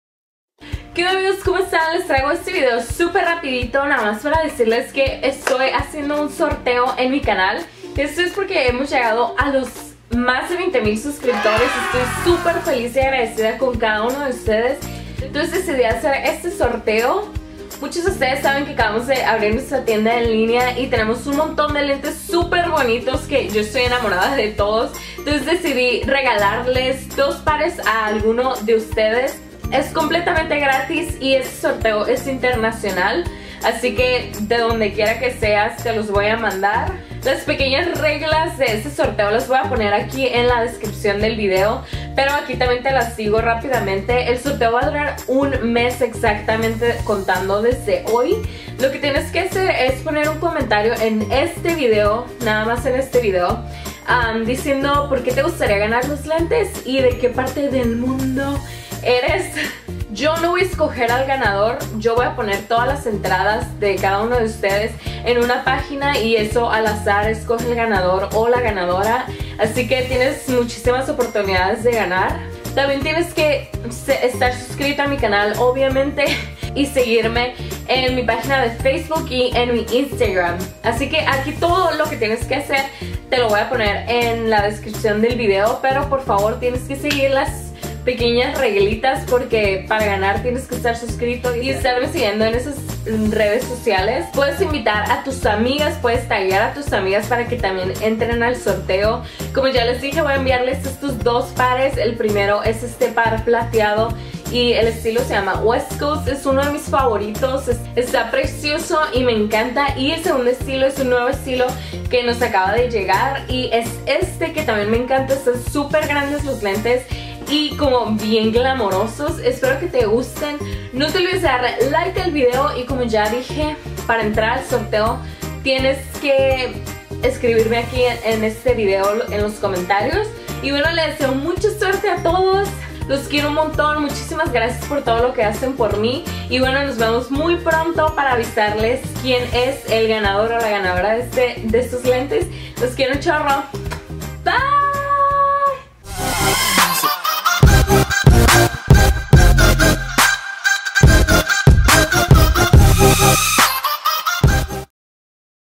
SORTEO INTERNACIONAL | ABIERTO!
Hola! Espero que puedas entrar a este pequeño sorteo de agradecimiento por ya ser mas de 20,000! Lo que voy a estar sorteando son estos lentes de sol de mi online store, son 2 pares super bellos!

LO QUE TIENES QUE HACER PARA ENTRAR AL SORTEO:
1. Comentar en este video por que quieres ganarte los lentes de sol y de donde eres (Ciudad y pais) Y TU EMAIL para contactarte!
2. Estar suscrito a este canal
3. Seguirme en mi pagina de LIKES de facebook
4. Seguirme en Instagram @ginnybc
5. Seguir el Instagram de mi tienda @ohhmysunnies
Y LISTO!

Puedes taggear a tus amigas para participar! Este es el primero de muchos sorteos!
MUCHA SUERTE! :)

Este video es acerca de: Sorteo, sorteo internacional, sorteo abierto, giveaway abierto, giveaway.